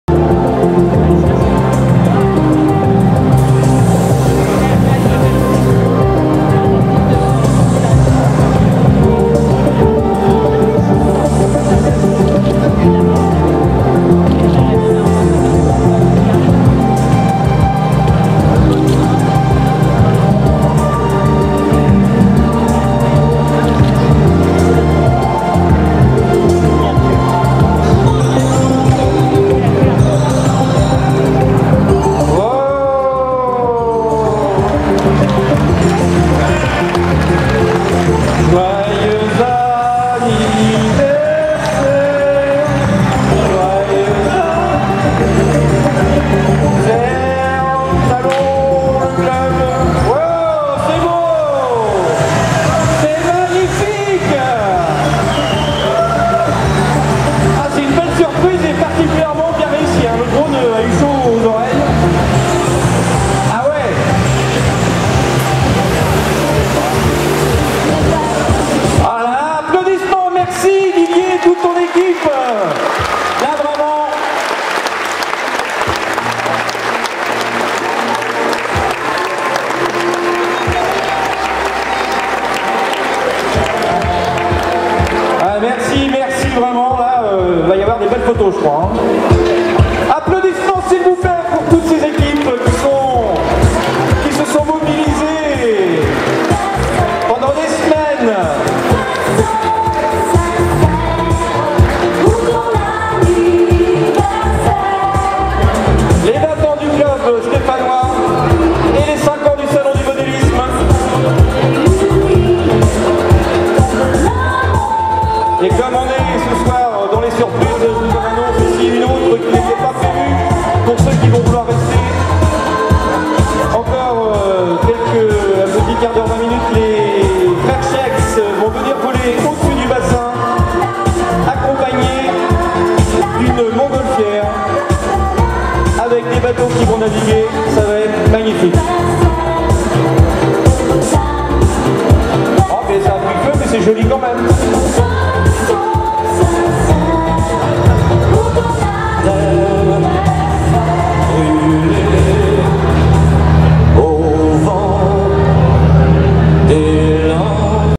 Salon du modélisme Argeles sur mer 2013
Pour les 20 ans de l'association de modélisme naval de St Estève
et les 5 ans du salon d'Argelès sur mer qui se déplace en 2014 à PERPIGNAN au Parc des expositions
Merci à Louis pour la vidéo